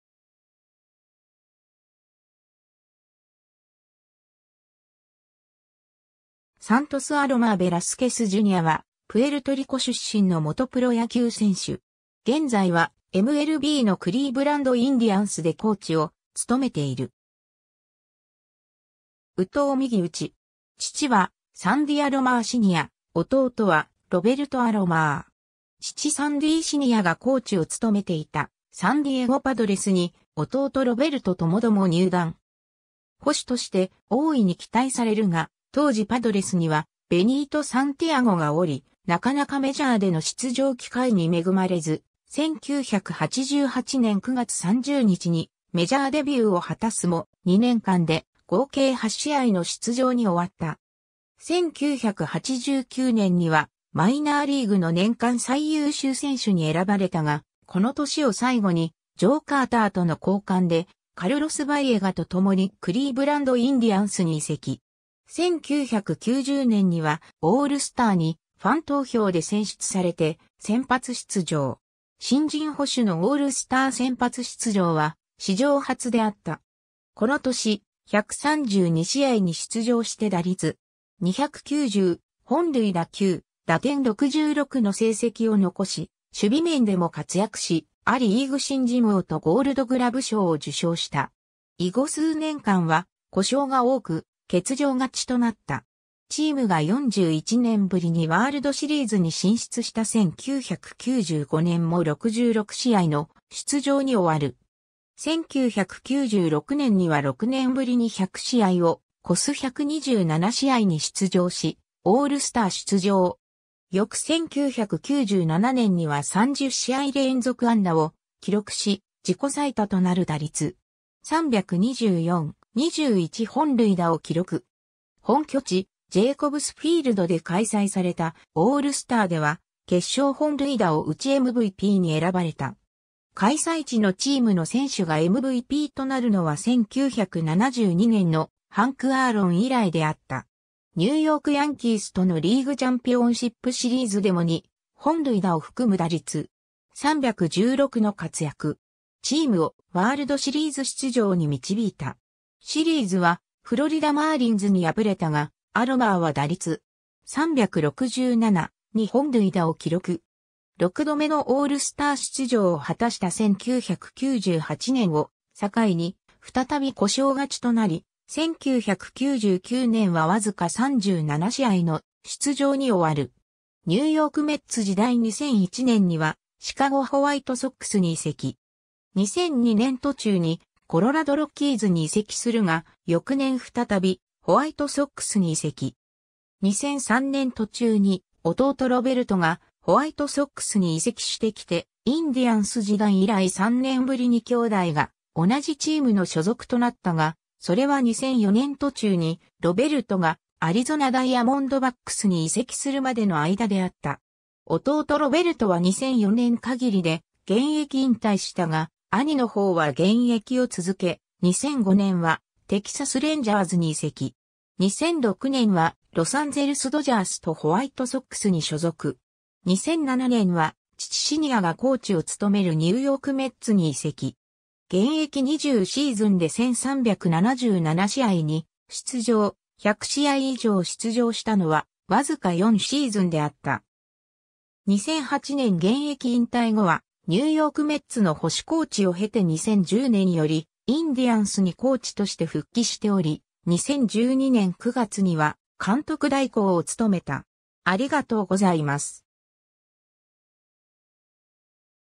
サンディー・アロマー・ジュニア
クリーブランド・インディアンス傘下の選手
サントス・アロマー・ベラスケス・ジュニア（Santos "Sandy" Alomar Velazquez, Jr. , 1966年6月18日 - ）は、プエルトリコ出身の元プロ野球選手。
現在はMLBのクリーブランド・インディアンスでコーチを務めている。
右投右打。
 父はサンディ・アロマー・シニア、弟はロベルト・アロマー。
 父サンディー・シニアがコーチを務めていたサンディエゴ・パドレスに弟ロベルト共々入団。
 捕手として大いに期待されるが、当時パドレスにはベニート・サンティアゴがおり、なかなかメジャーでの出場機会に恵まれず、1988年9月30日にメジャーデビューを果たすも、2年間で合計8試合の出場に終わった。
 1989年にはマイナーリーグの年間最優秀選手に選ばれたが、この年を最後にジョー・カーターとの交換で、カルロス・バイエガと共にクリーブランド・インディアンスに移籍。
 1990年にはオールスターにファン投票で選出されて先発出場。
 新人捕手のオールスター先発出場は史上初であった。
 この年、132試合に出場して打率.290、本塁打9、打点66の成績を残し、守備面でも活躍し、ア・リーグ新人王とゴールドグラブ賞を受賞した。
 以後数年間は故障が多く、欠場がちとなった。
 チームが41年ぶりにワールドシリーズに進出した1995年も66試合の出場に終わる。
 1996年には6年ぶりに100試合を越す127試合に出場し、オールスター出場。
 翌1997年には30試合連続安打を記録し、自己最多となる打率.324、21本塁打を記録。
 本拠地ジェイコブス・フィールドで開催されたオールスターでは、決勝本塁打を打ちMVPに選ばれた。
開催地のチームの選手がMVPとなるのは1972年のハンク・アーロン（ブレーブス）以来であった。
 ニューヨーク・ヤンキースとのリーグチャンピオンシップシリーズでも2本塁打を含む打率.316の活躍。
 チームをワールドシリーズ出場に導いた。
シリーズはフロリダ・マーリンズに敗れたが、アロマーは打率.367、2本塁打を記録。
 6度目のオールスター出場を果たした1998年を境に、再び故障がちとなり、1999年はわずか37試合の出場に終わる。
 ニューヨーク・メッツ時代 (2007年3月11日) 2001年にはシカゴ・ホワイトソックスに移籍。
 2002年途中にコロラド・ロッキーズに移籍するが翌年再びホワイトソックスに移籍。
 2003年途中に弟ロベルトがホワイトソックスに移籍してきて、インディアンス時代以来3年ぶりに兄弟が同じチームの所属となったが、それは2004年途中にロベルトがアリゾナ・ダイヤモンドバックスに移籍するまでの間であった。
 弟ロベルトは2004年限りで現役引退したが、兄の方は現役を続け、2005年はテキサス・レンジャーズに移籍。
 2006年はロサンゼルス・ドジャースとホワイトソックスに所属。
 2007年は父シニアがコーチを務めるニューヨーク・メッツに移籍。
現役20シーズンで1,377試合に出場、100試合以上出場したのはわずか4シーズンであった。
 2008年現役引退後はニューヨーク・メッツの捕手コーチを経て2010年よりインディアンスにコーチとして復帰しており、2012年9月には監督代行を務めた。